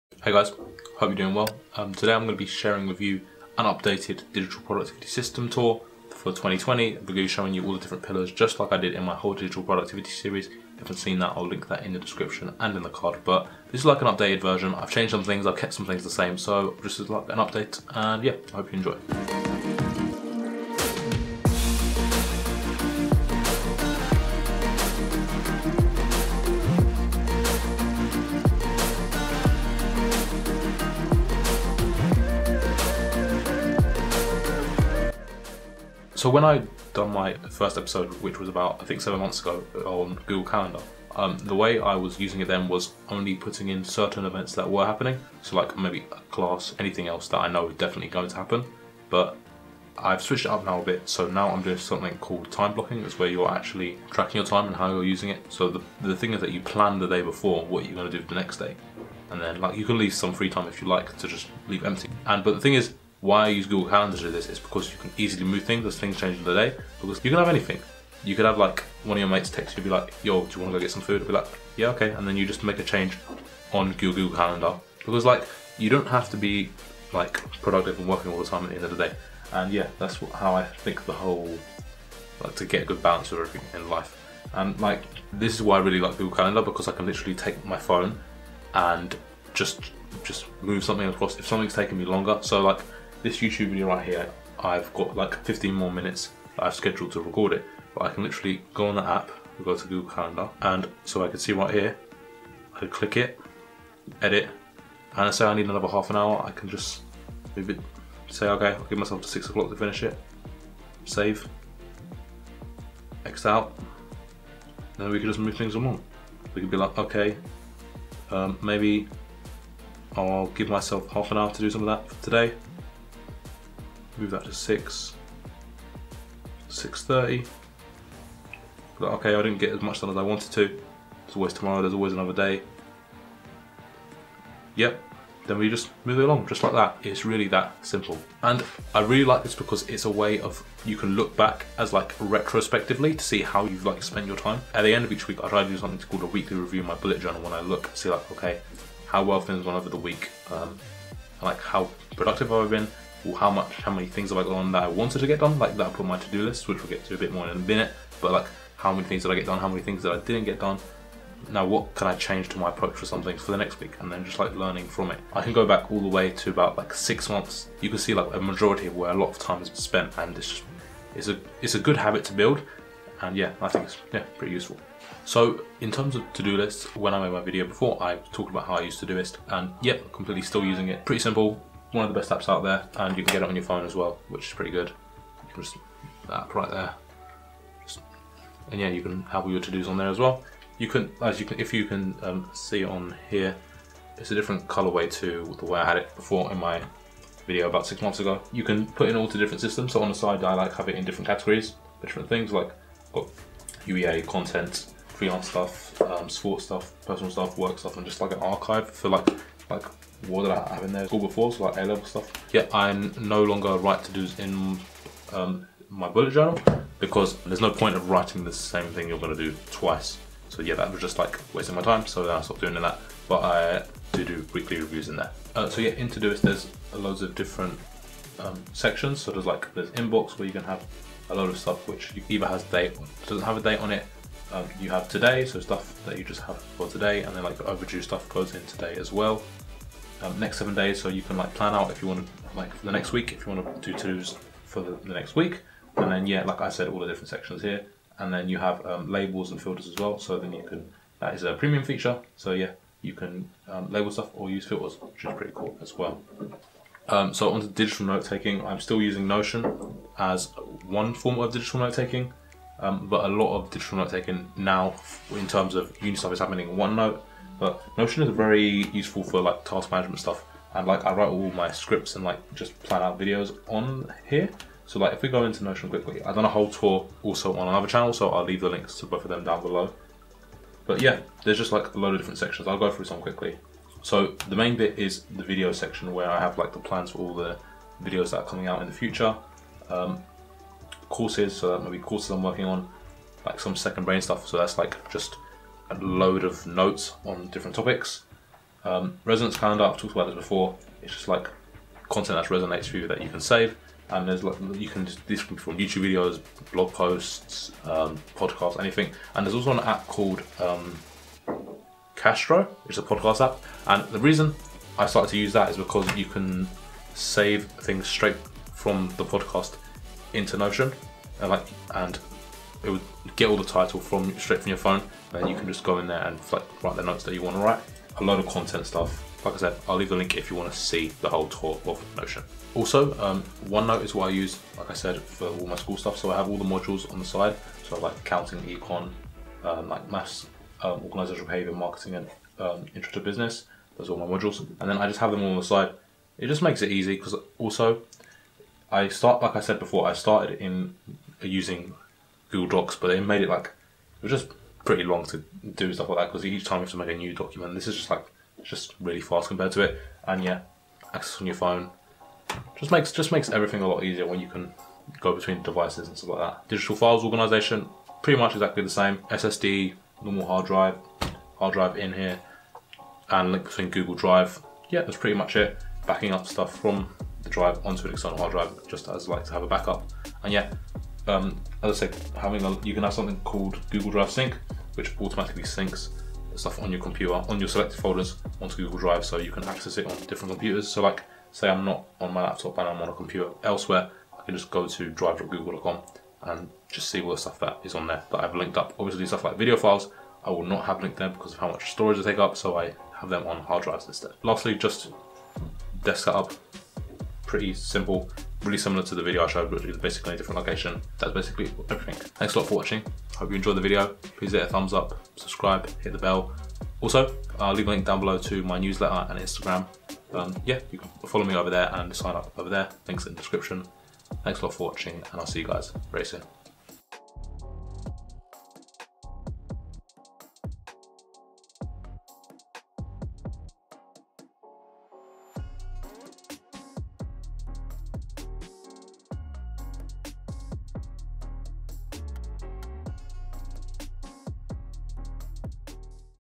My Updated Productivity System For 2020: Digital Productivity Ep 7
Here is an update to my digital productivity system for 2020!

After making my digital productivity series somethings have stayed the same, however, there have been some changes and a big shift in terms of how I am using my calendar. Hope you enjoy!

What's your favourite pillar of your approach to digital productivity?

Where I get the majority of my music (awesome for creators!